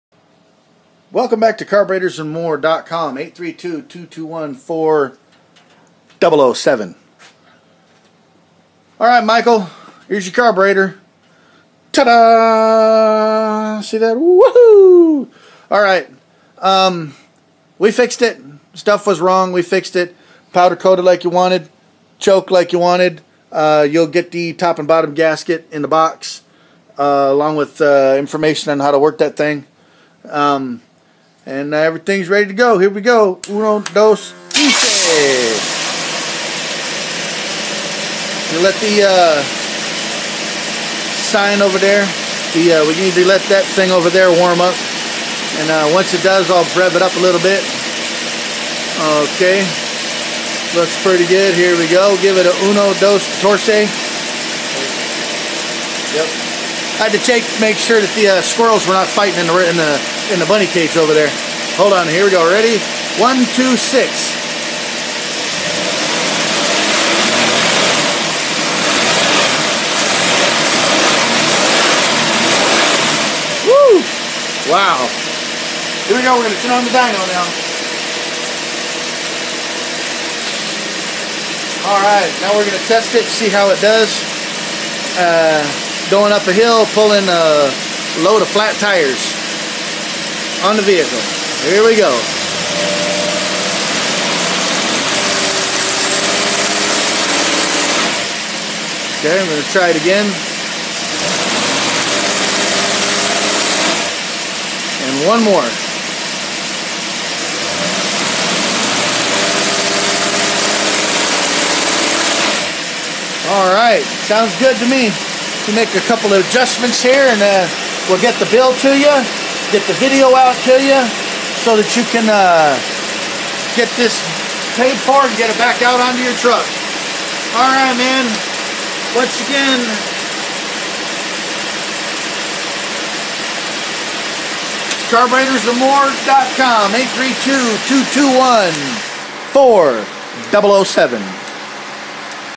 Michael Autolite carb
This is an Autolite carb that is being tested on a running engine.